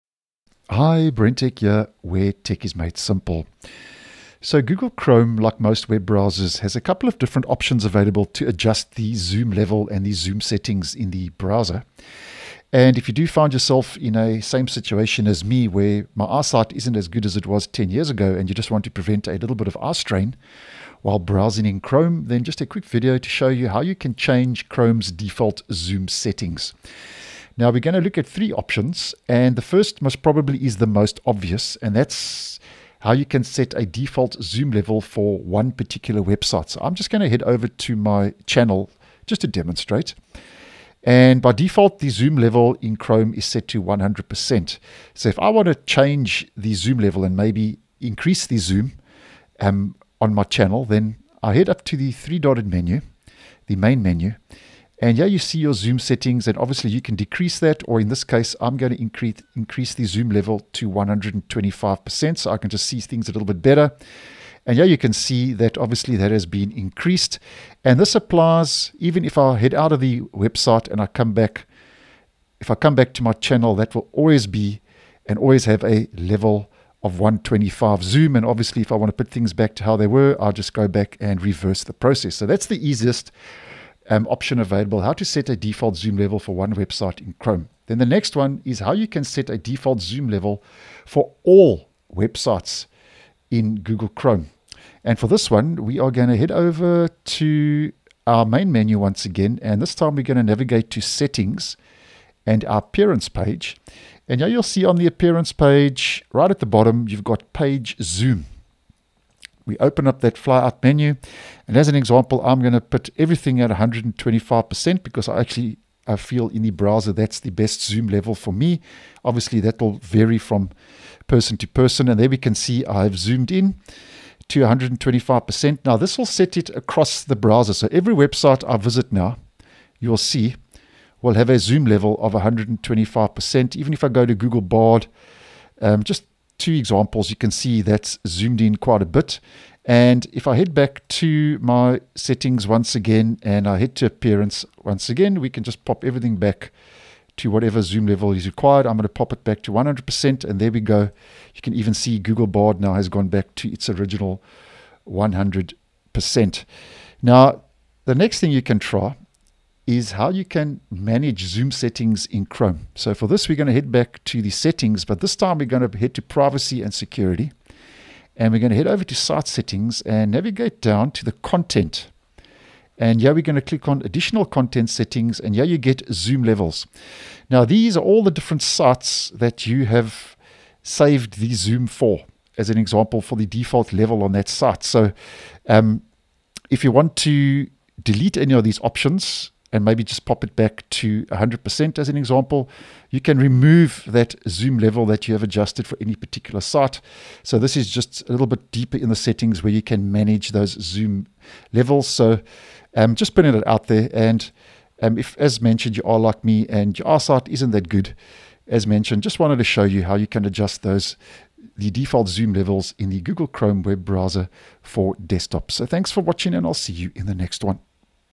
How to Change Google Chrome's Default Zoom Settings
Whether you want to increase the text to prevent eye strain or need to decrease the size of a website, Chrome has a couple of options available to help you out.